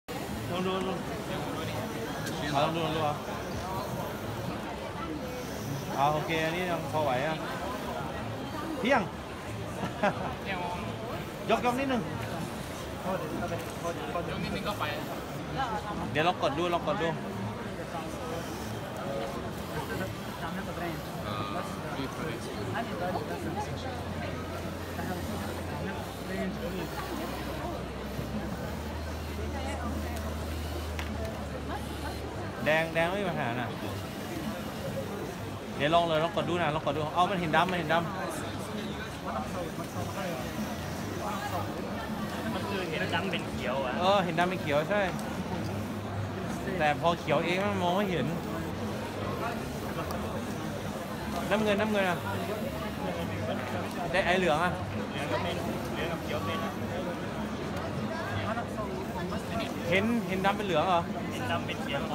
WRO 2019 - REGULAR SENIOR CATEGORY - THAILAND TEAM
Thầy gửi tới các bạn hình ảnh từ nhà thi đấu Olympic Sport Park - Gőry. Bài thi của đội Thái Lan trong ngày chạy thử sa bàn thứ nhất.